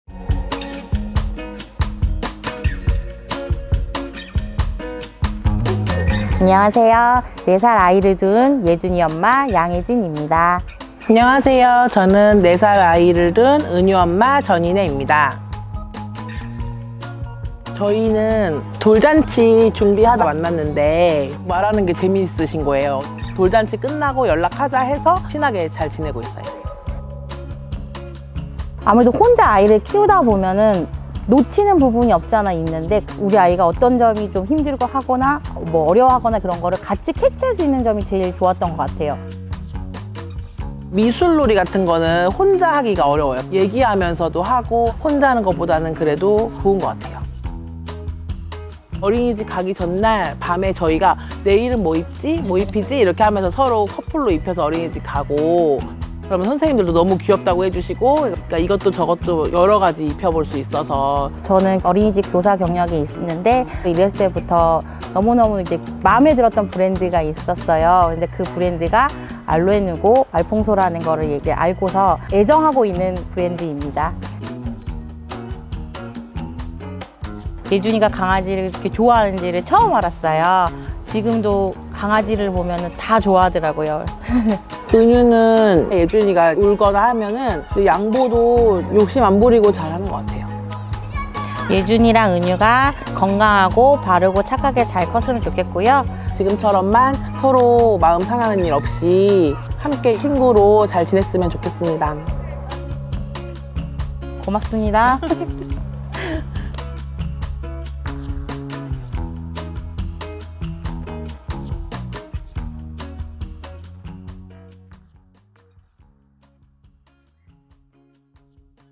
알로앤루, 알퐁소 "함께해요" 캠페인 #1.친구와 함께 | 둘이라서 즐겁고, 함께라서 든든하다!
알로앤루, 알퐁소 "함께해요" 캠페인 1.친구와 함께

둘이라서 즐겁고, 함께라서 든든한
4살 친구 예준 & 은유와 엄마들의 이야기를 만나 보세요!

00:17 우리 이렇게 친해졌어요~
00:28 함께육아의 좋은 점!!
00:40 즐거운 미술놀이 타임
01:02 알로앤루, 알퐁소를 좋아하게 된 계기

아이들의 건강한 미래와
가족의 행복한 문화를 함께 만들어 갑니다♥